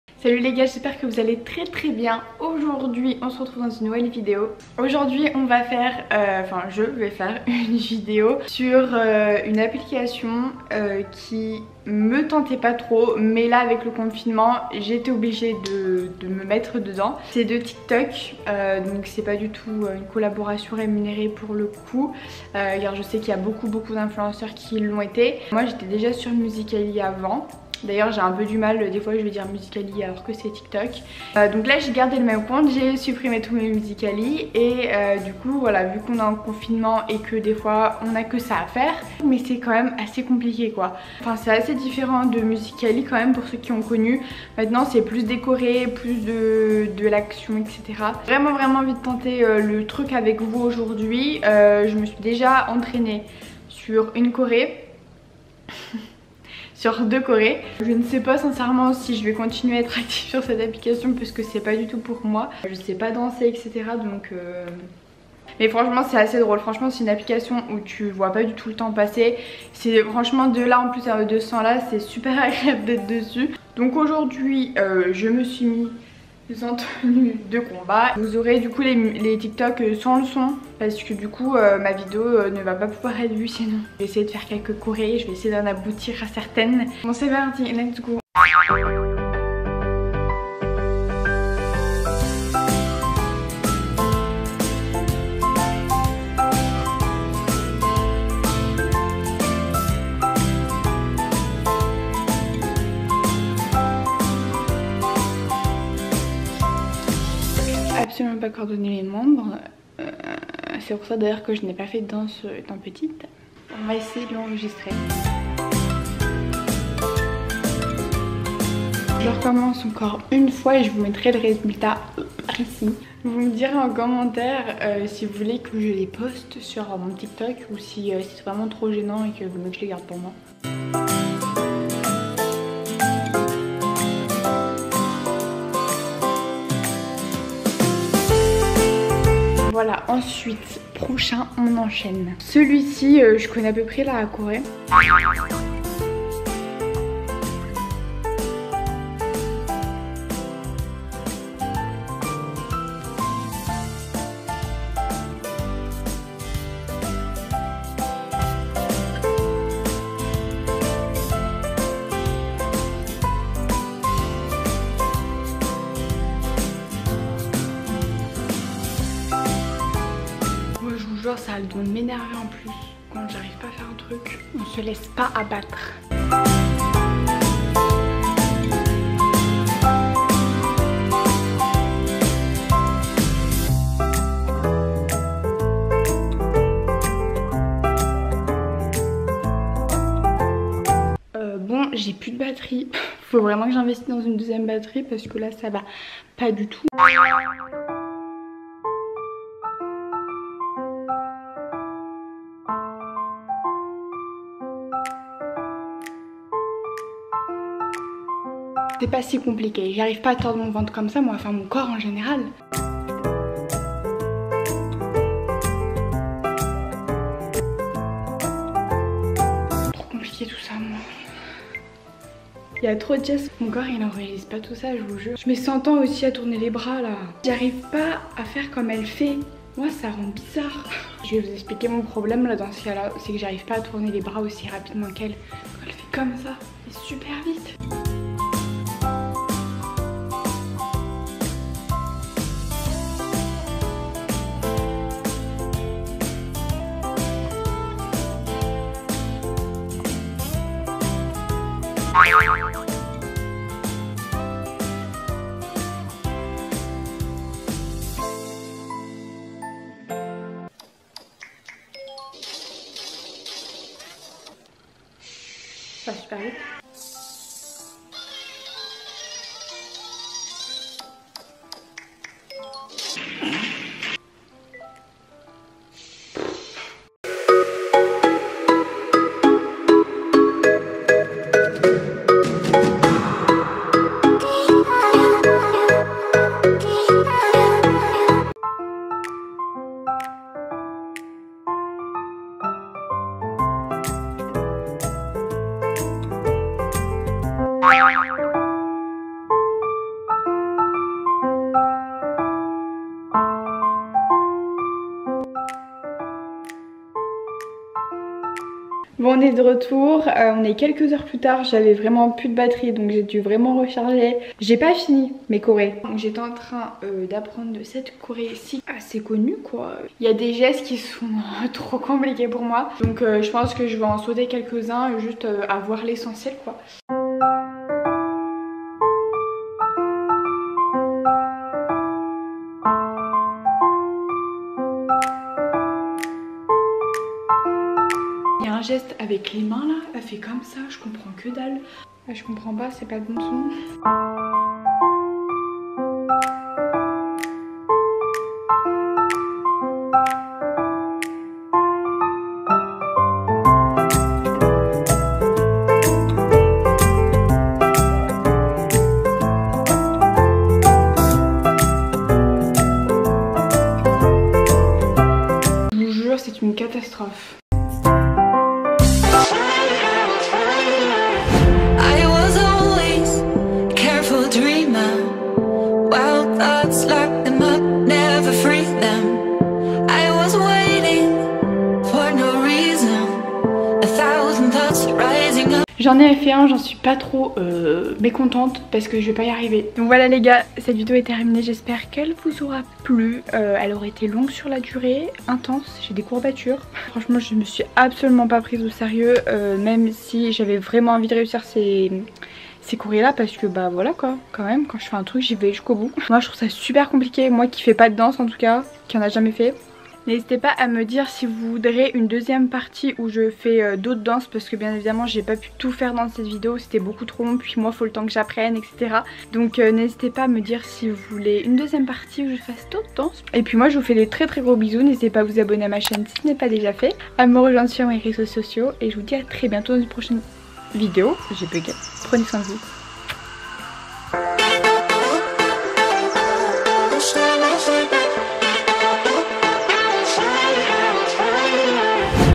J'ESSAYE DE FAIRE DES TIK TOK (et c'est pas simple) ♡ - Juste Emma
↓ Déroule la barre d'infos ↓

j'espère que tout va bien pour vous.
aujourd'hui je m'attaque à l'application Tik Tok anciennement Musicaly.
Omg c'est pas simple, j'ai eu du mal mais j'ai essayé!!
Mon compte Tik Tok : justeemma_
Vous voulez une deuxième partie ???

♡ Ce que je porte dans cette vidéo :
♡ HAUT⟩
 ZARA
♡ BAS ⟩
STRADIVARIUS
♡ BIJOUX ⟩

• PAGE FACEBOOK : JUSTE EMMA

• JE FILME AVEC ⟩ CANON g7x

Emma 💋